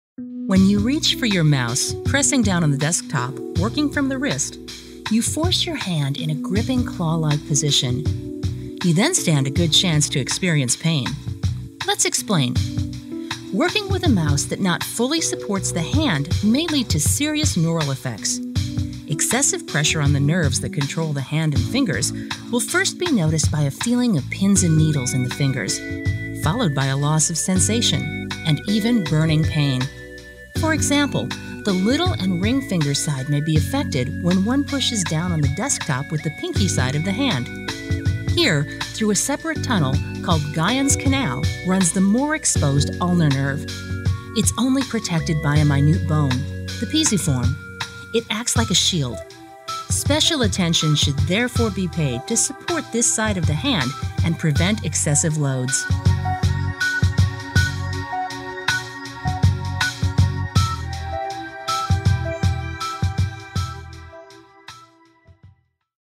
HandShoe Mouse And Gyons Canal
Some people may not be aware that the pain in their hand, when using a standard computer mouse or a vertical mouse, is caused by pushing down the pinky side of the hand.This may have an effect on the so called Gyons Canal, as shown in this video.
With a HandShoe Mouse this will not happen, because your hand is fully supported.